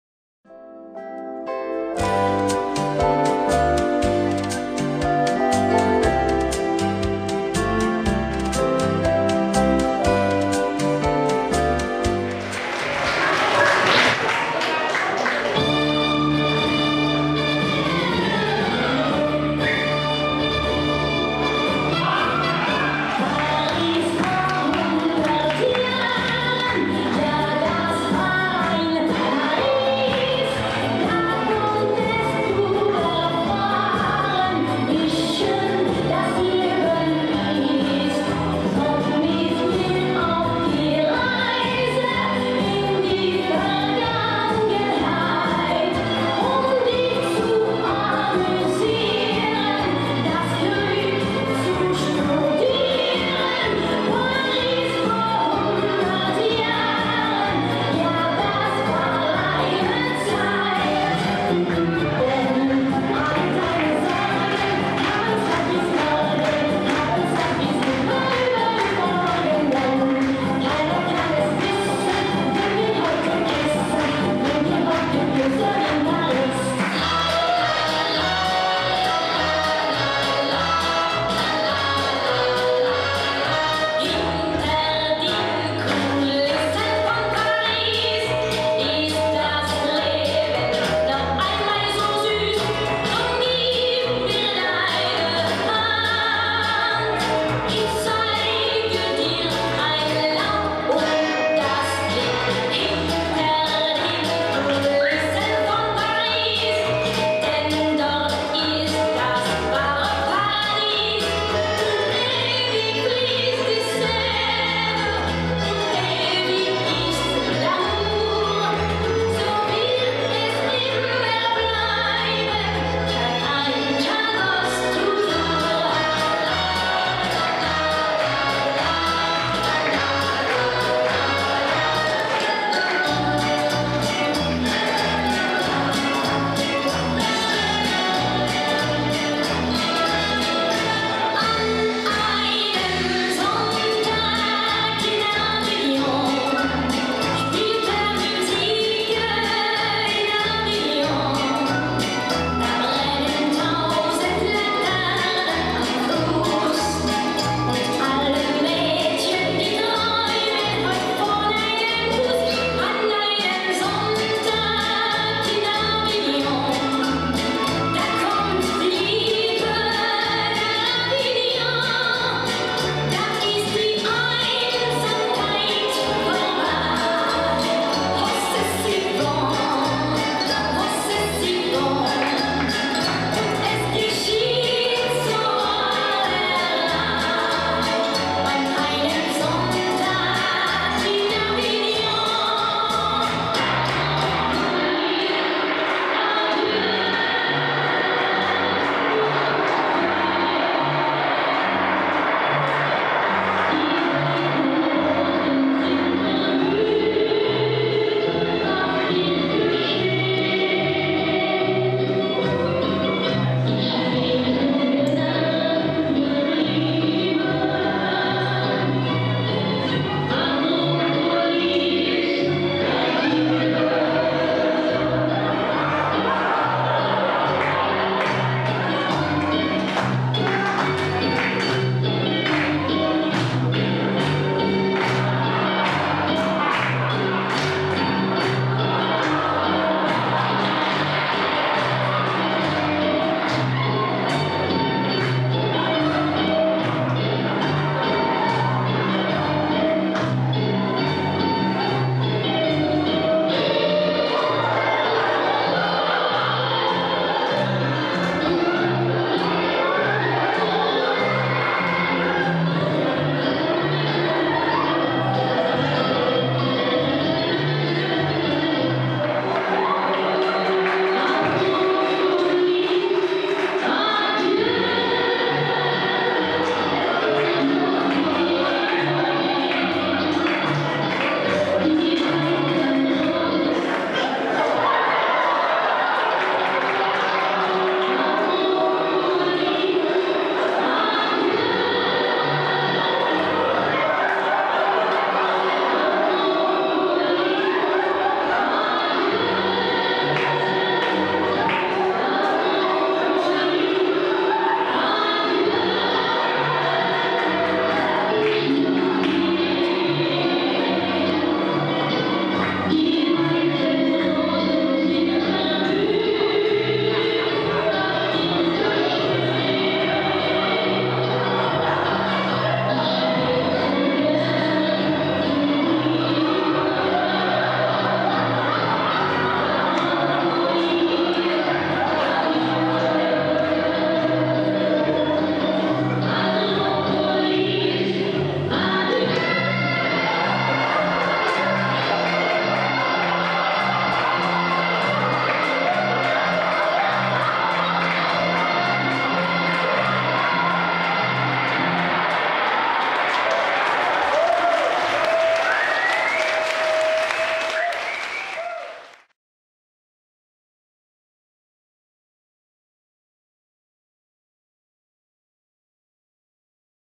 Paris Persiflage
Iedere 2e zondag van de maand is er een 50+ soos voor homo's lesbo's enz. ook de buren zijn welkom en anderen. Dit vond plaats in Dienstencentrum Copernicus in de Daguerrestraat 16 te Den Haag. Naast het ontmoeten worden er meestal 3 playbackshows gegeven door de vrijwilligers en soms ook de gasten. Dit keer is het onze gasten.